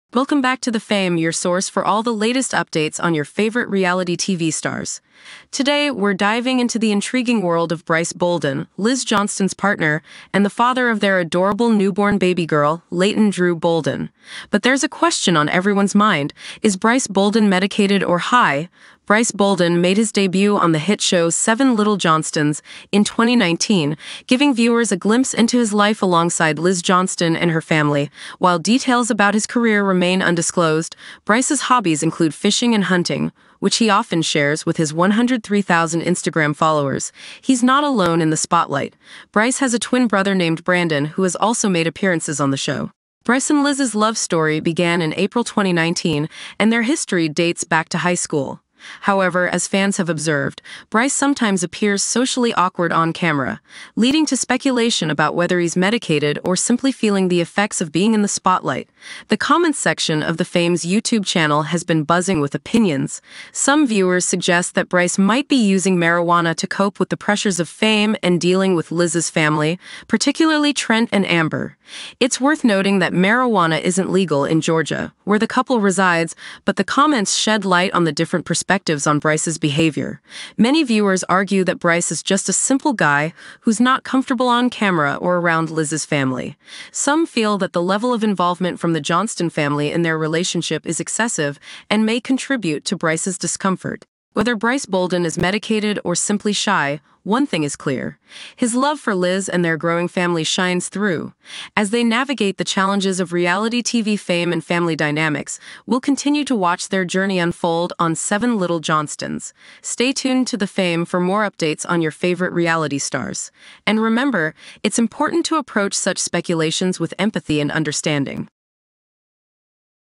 7 Little Johnstons Liz Johnston’s Partner Brice Bolden is Medicated or High?
Join us as we delve into the enigmatic world of Brice Bolden, partner to Liz Johnston and a star of "7 Little Johnstons." Is Brice medicated, or is he simply a laid-back, shy guy? Dive into the discussion, explore fan opinions, and gain insights into Brice's life, hobbies, and his relationship with Liz's family. Stay updated on all the latest reality TV drama with TheFame.